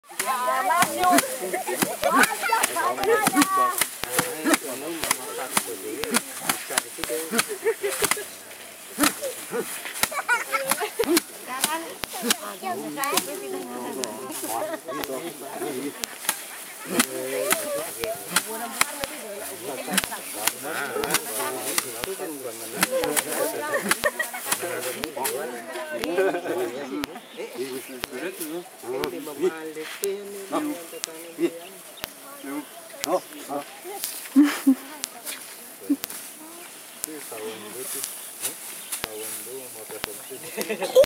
JP bat le riz à Madagascar
Sur la route d'Antsirabe à Ambositra